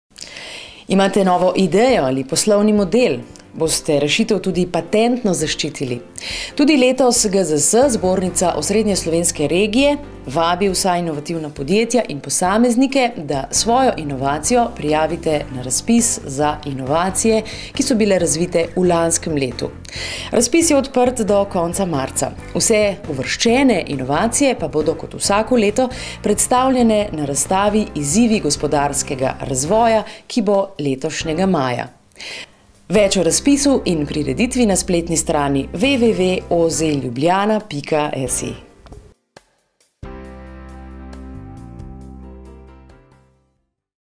Izzivi gospodarskega razvoja 2013: prijavite se na razpis!
GZS - Zbornica osrednjeslovenske regije vabi k prijavam na razpis za zbiranje prijav za podelitev priznanj in diplom inovacijam na območju Zbornice osrednjeslovenske regije za leto 2012. Več